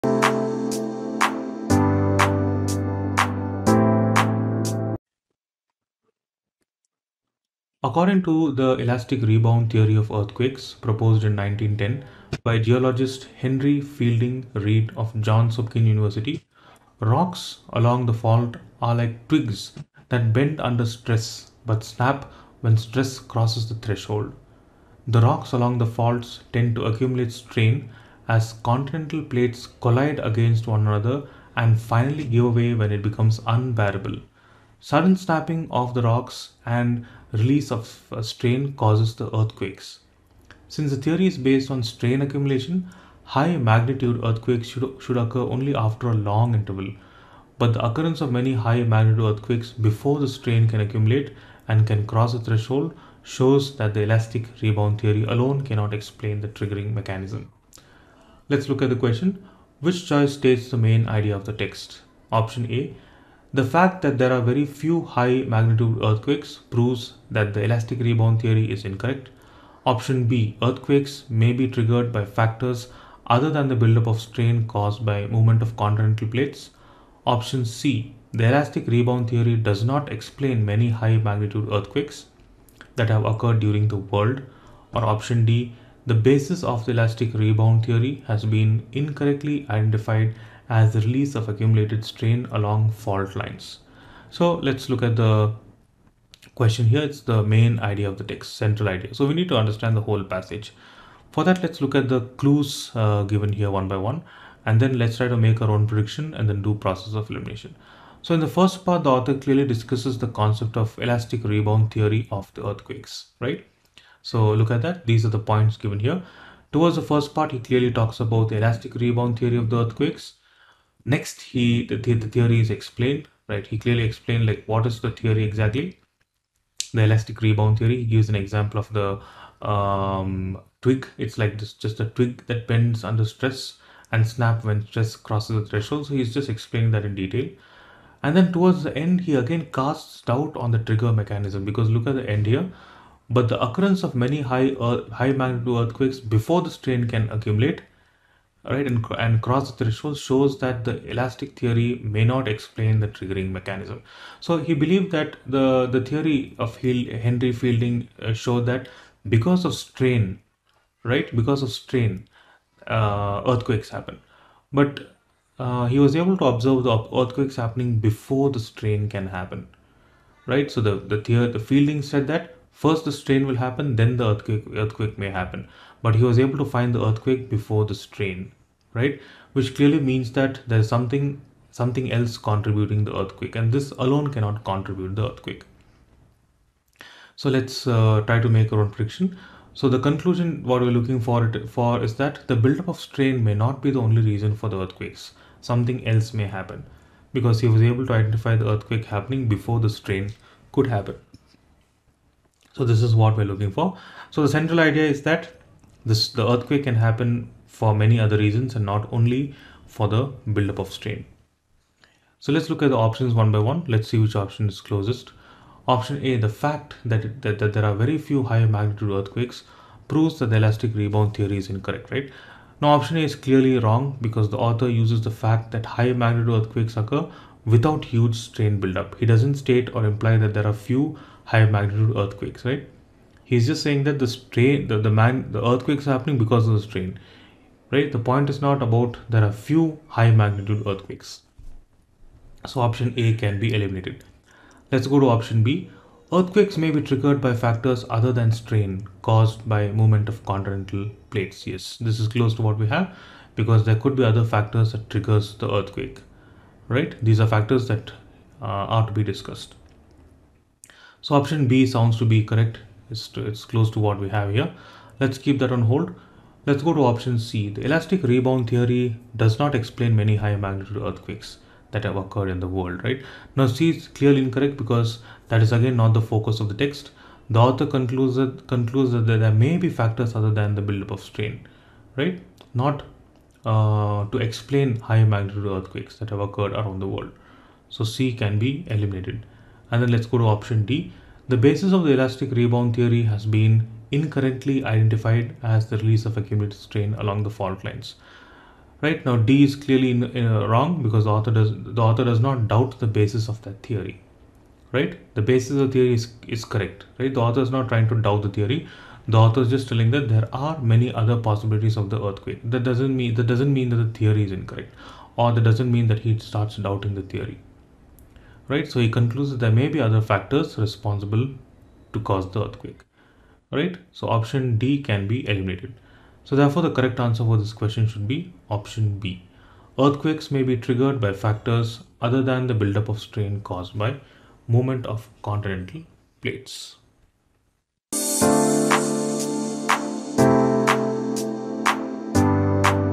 ACT Reading Prep On: Main Idea Session
About Video:

Welcome to the ACT Reading: Main Idea session. We have tried to ease things for you and hope you all find it helpful.

Need help with ACT Preparation?

Reach out to Team ReWiser!!!

🌐 You can check out these links for more information about the Rewiser